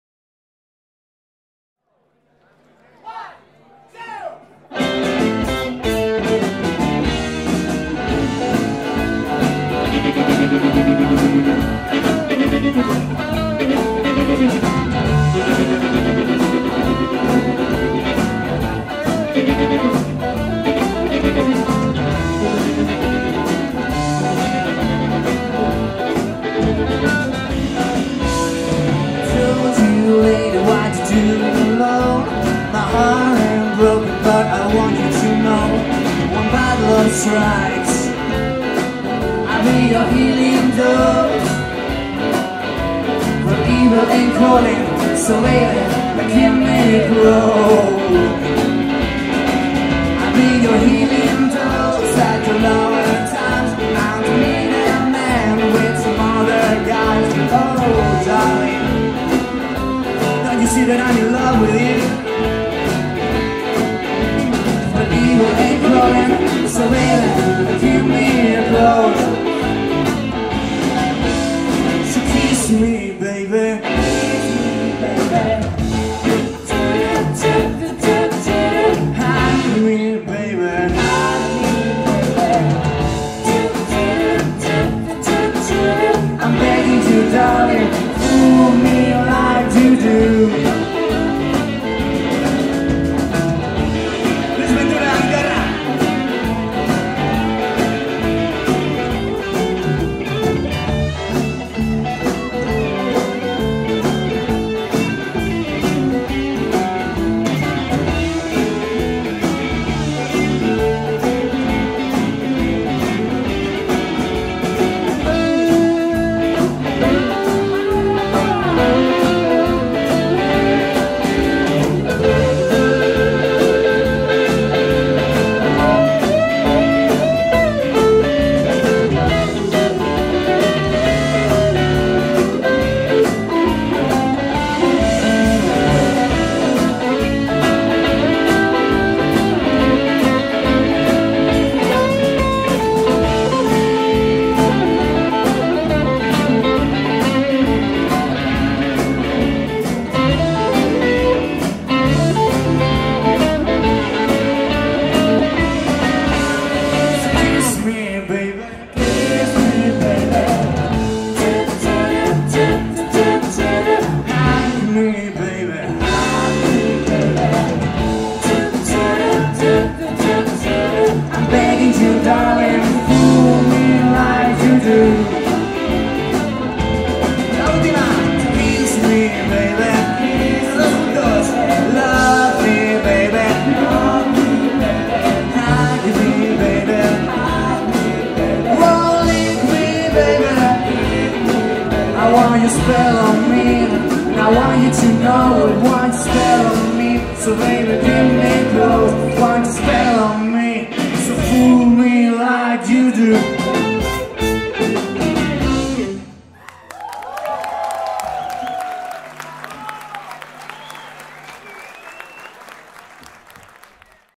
Manuel de la Cueva & The Breeze (2) - Semifinal concurso Ambar Z Music 2018
Actuación de Manuel de la Cueva & The Breeze en la primera semifinal del concurso Ambar Z Music en Las Armas de Zaragoza, el día 10 de mayo de 2018.